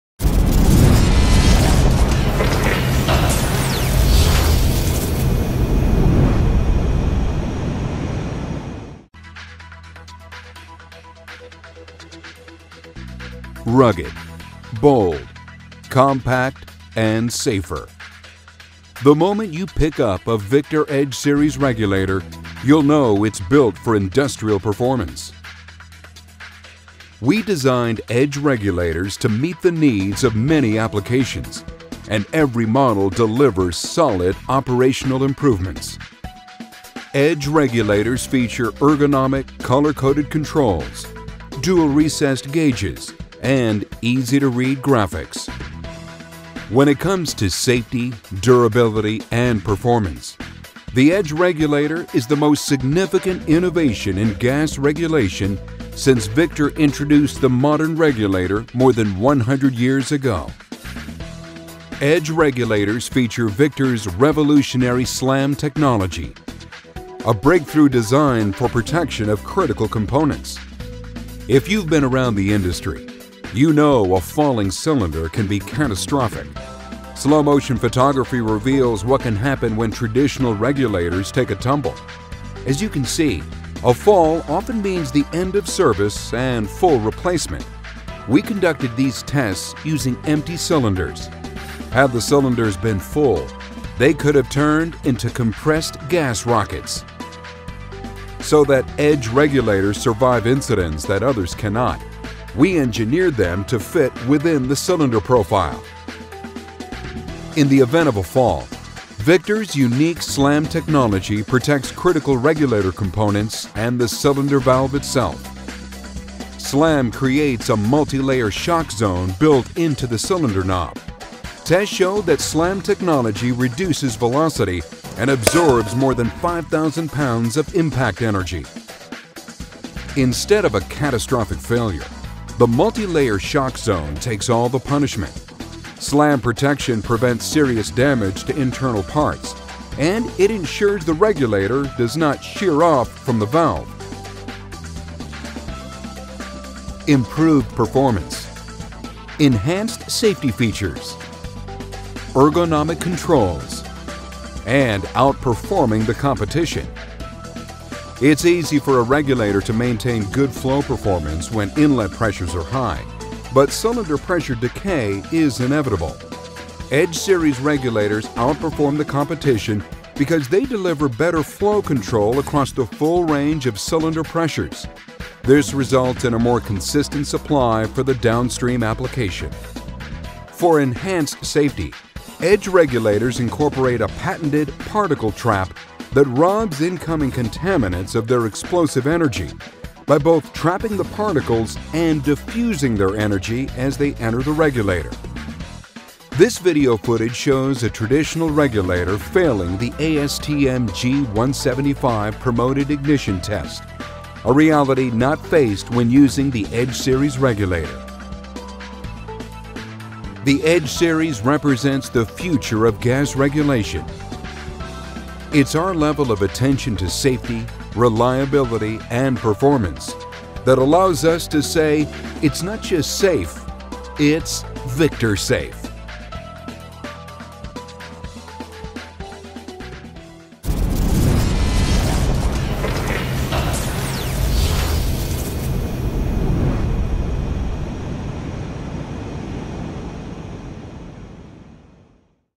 Victor G-Series G350 Single Stage Acetylene Regulator Heavy-Duty Model# 0781-9430
This heavy-duty Victor® G-Series G350 Single Stage Acetylene Regulator offers 15 psig pressure delivery and is comfortable to use. The regulator features easy-to-read high contras...